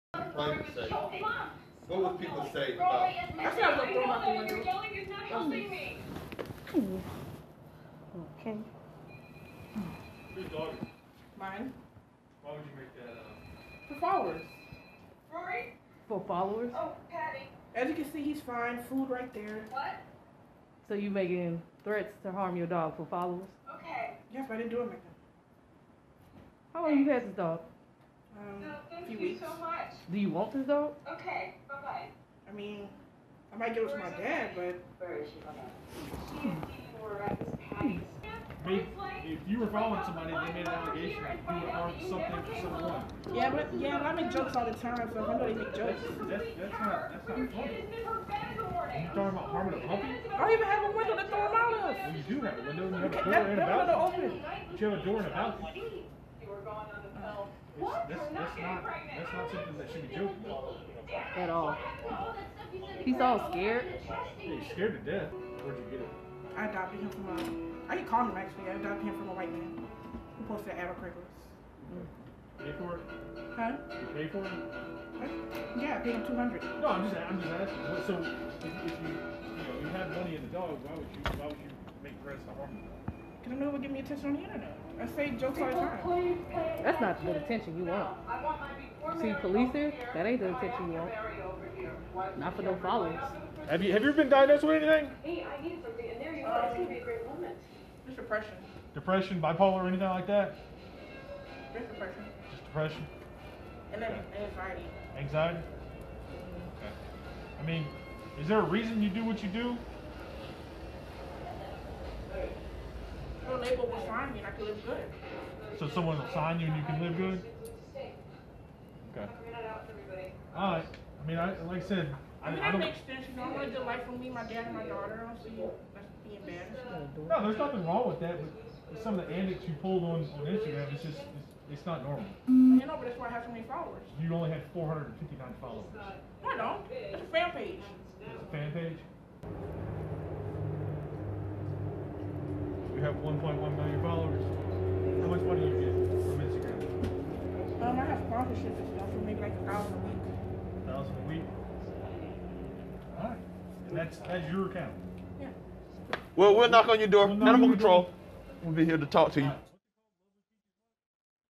Lovely Peaches visited by the police - REAL BODYCAM FOOTAGE (HD)
Brittany Johnson aka Lovely Peaches gets a visit from the cops after threatening her dog on social media.